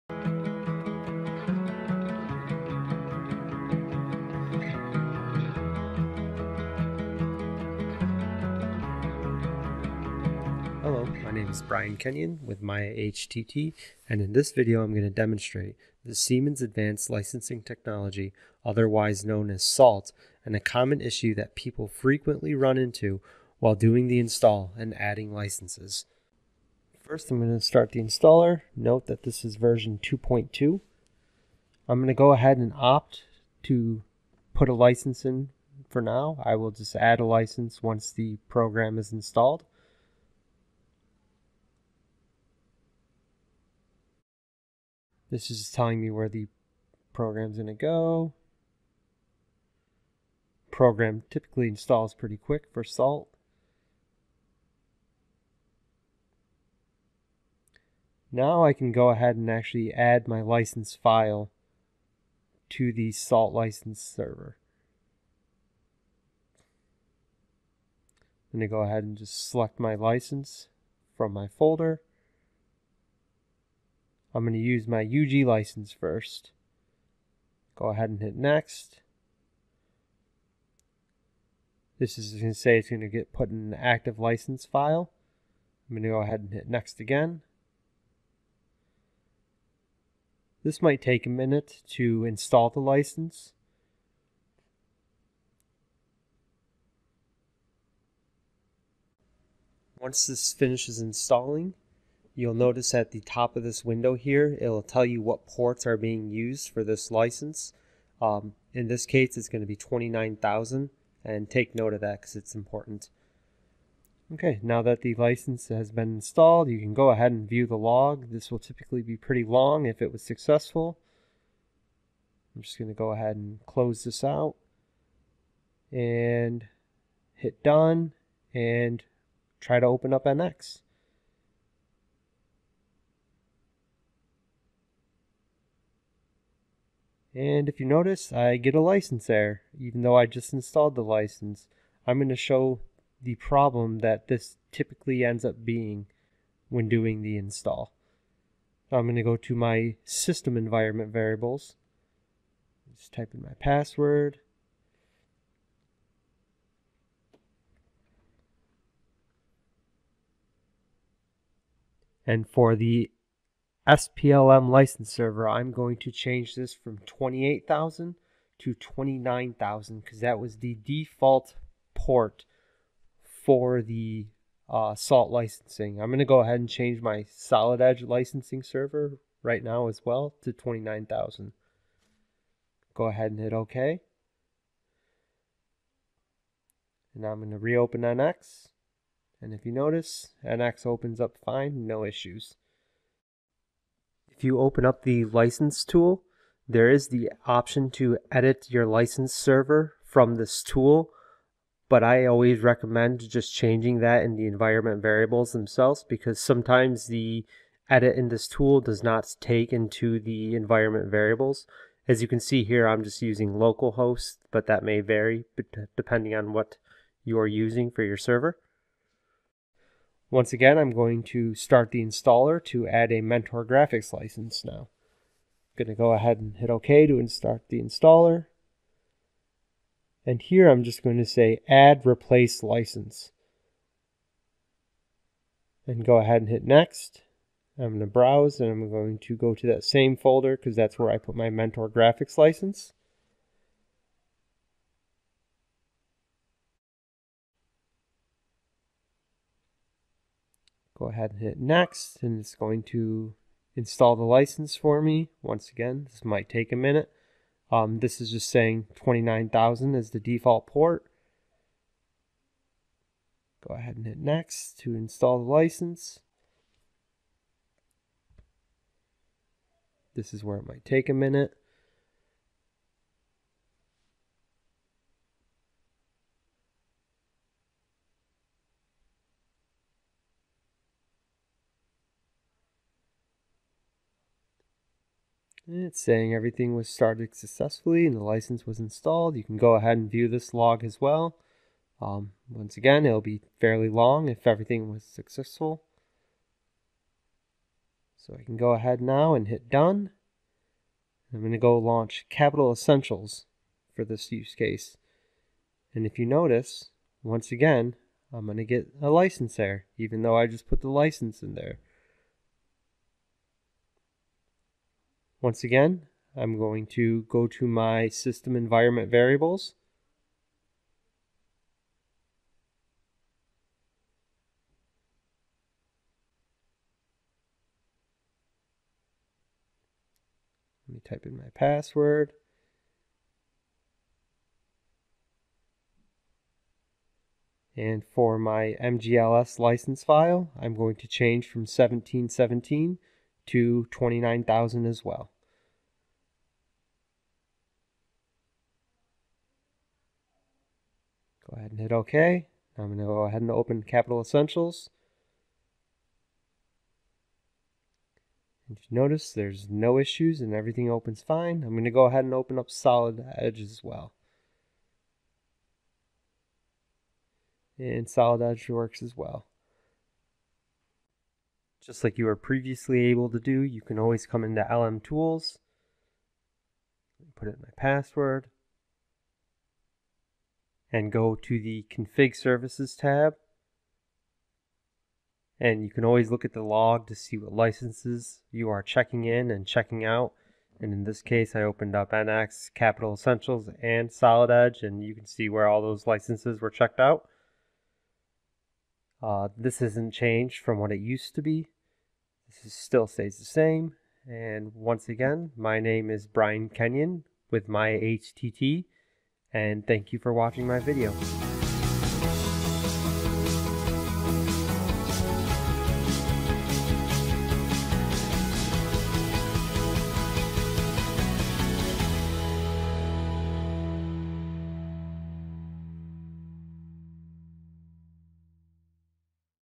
Common Port Issue For Adding Licenses With Siemens License Server
Common Port issue when adding a license file when using the Siemens Licensing Server